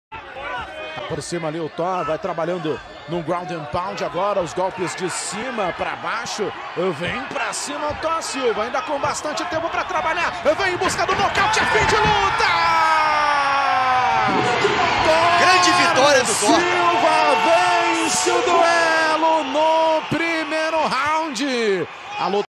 Wanderlei Silva Son Thor Won His First MMA Debut With TKO In First Round
Thor Silva the sun of the legendary axe murdered wanderlei Silva. Won his first MMA Fight With TKO in the first round